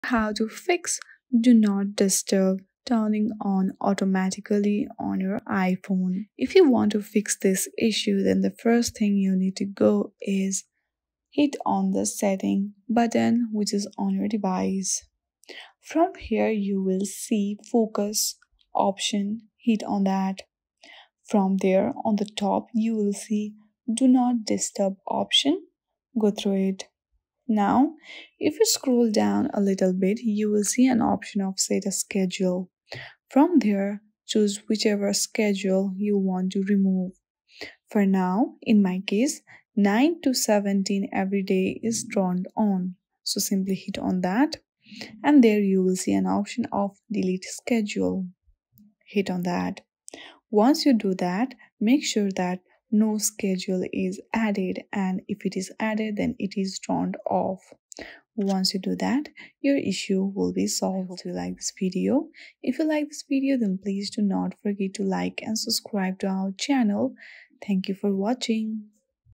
How to Fix Do Not Disturb Turning On Automatically on iPhone
Is your iPhone's Do Not Disturb mode turning on unexpectedly? Frustrated by constant interruptions? Don't worry, we've got you covered! In this video, Tiki Taka Tech will guide you through simple steps to fix the Do Not Disturb issue on your iPhone. Learn how to disable automatic Do Not Disturb, troubleshoot common problems, and enjoy uninterrupted notifications.

00:00:00 - Introduction
How to fix the "Do Not Disturb" turning on automatically on your iPhone.

00:00:05 - Open Settings
Hit the Settings button on your device.

00:00:10 - Access Focus Settings
Select the Focus option in the Settings menu.

00:00:15 - Navigate to Do Not Disturb
Hit the Do Not Disturb option at the top of the Focus menu.

00:00:20 - Set a Schedule
Scroll down to find the "Set a Schedule" option.

00:00:25 - Remove Schedule
Choose the schedule you want to remove and select "Delete Schedule."

00:00:40 - Ensure No Schedules are Active
Make sure no schedules are added or turned on.

00:00:50 - Conclusion